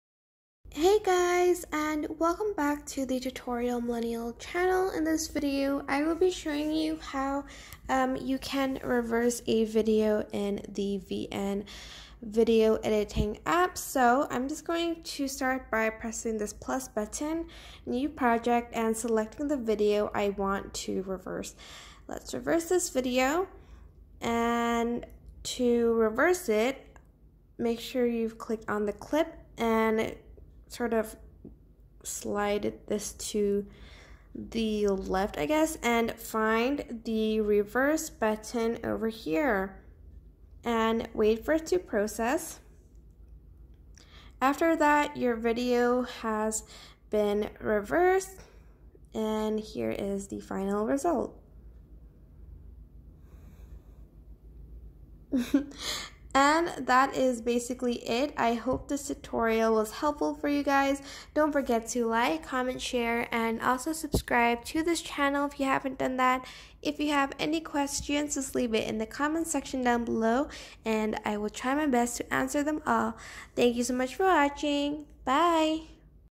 How To Reverse Your Video In VN
Want to reverse your video? Here's how to do that in VN!